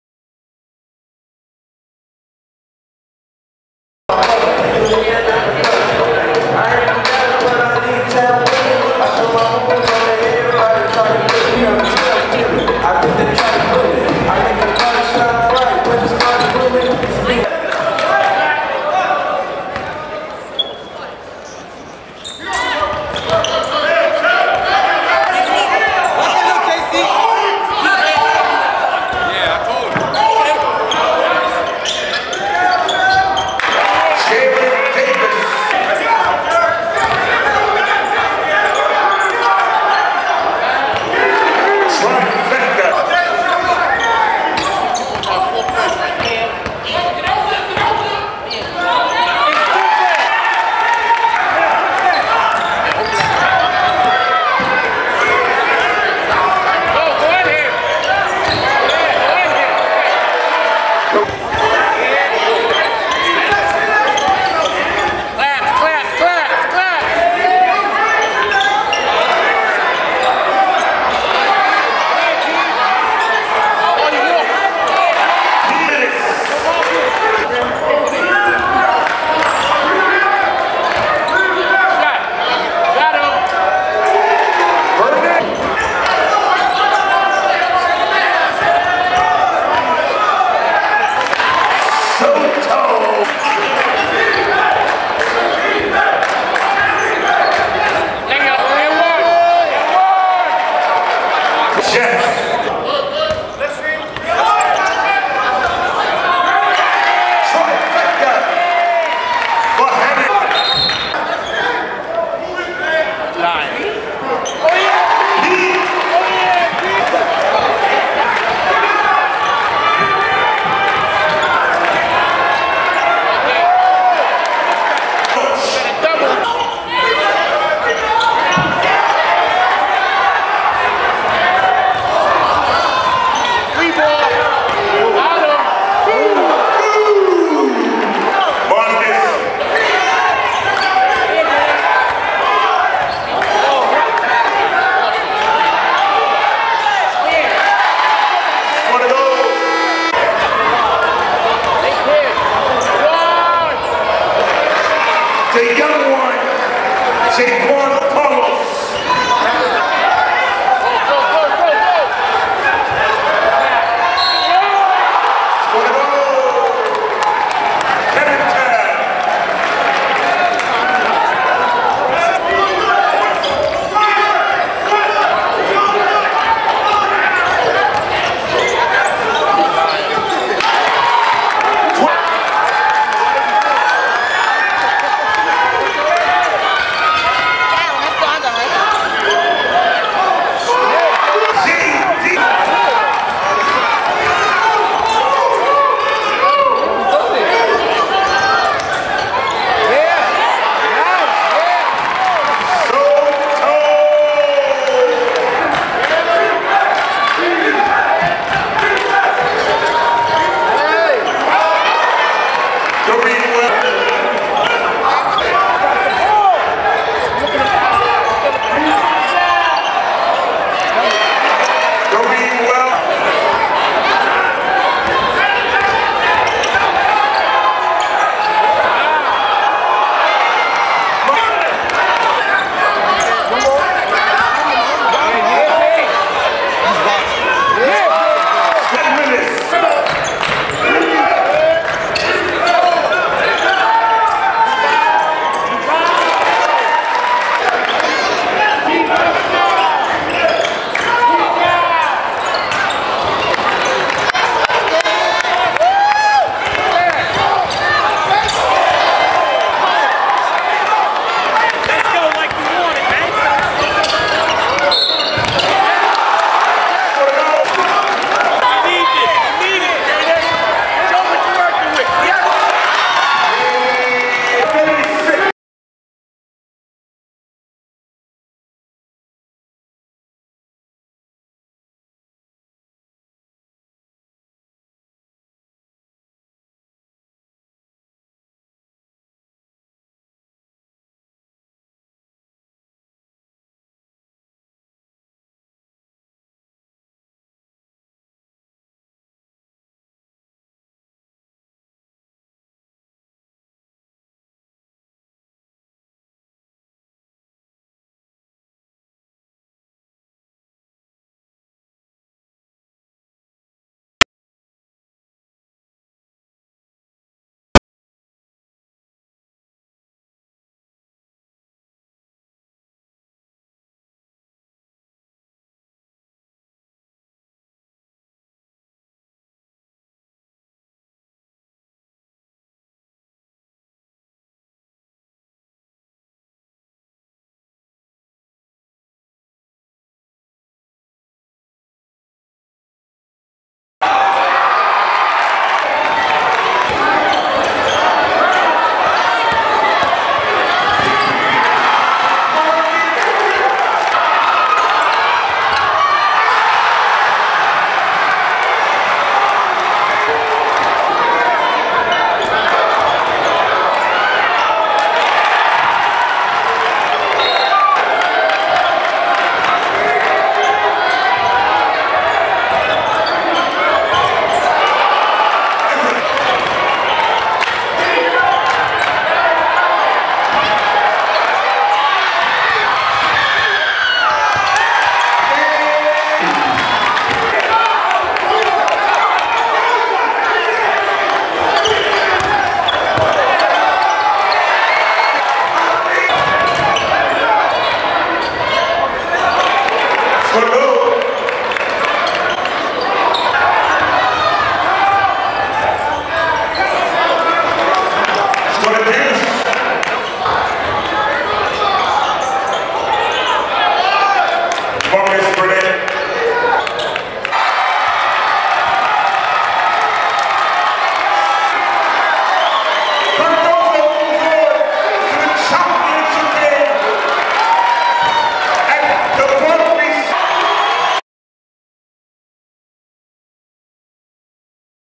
PSAL Semifinal: Jefferson vs. Cardozo
Highlights of Jefferson vs. Cardozo in the PSAL semifinals at Carnesecca Arena on March 11, 2018.